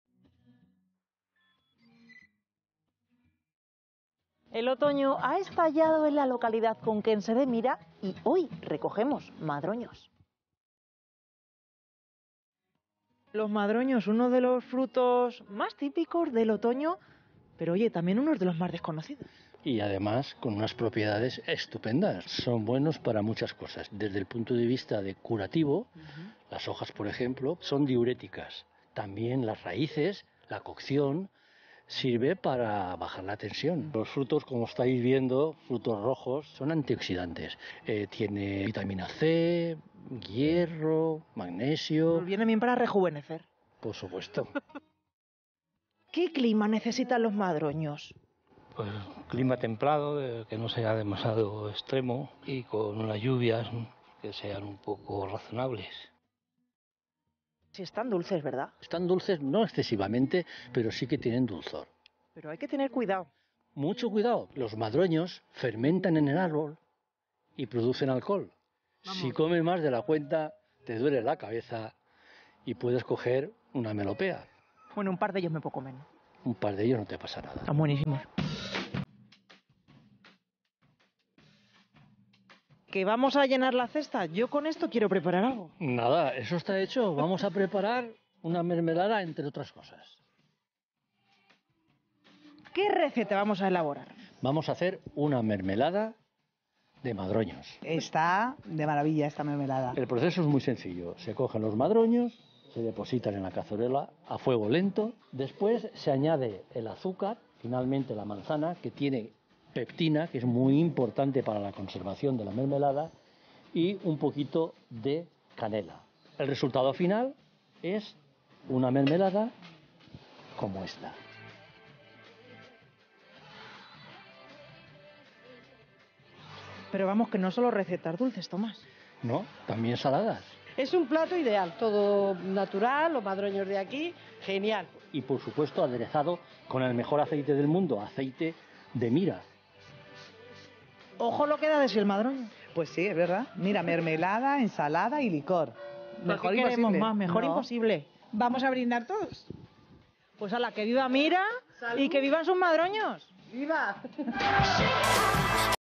Con el estallido del otoño recogemos madroños en Mira | Ancha es Castilla-La Mancha
Típico del otoño es el madroño, ya maduran en muchos bosques de nuestra región. En el municipio conquense de Mira han preparado unas recetas deliciosas con los madroños.